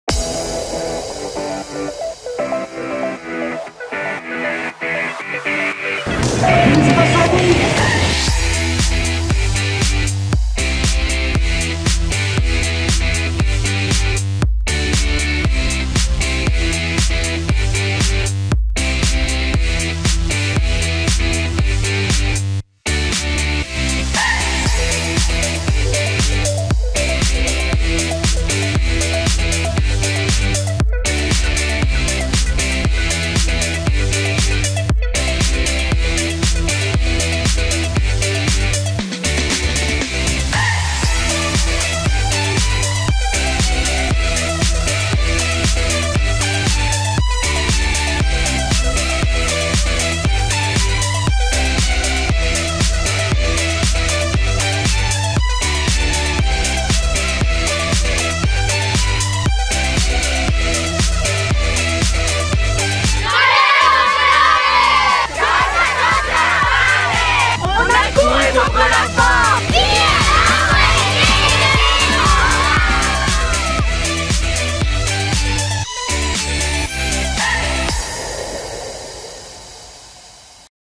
Carrera contra el Hambre | Más de 26 años corriendo para salvar vidas.
La Carrera contra el Hambre es un proyecto educativo que promueve valores y competencias de manera divertida y saludable.
Solo en España han participado más de 1.500 centros educativos y 700.000 estudiantes.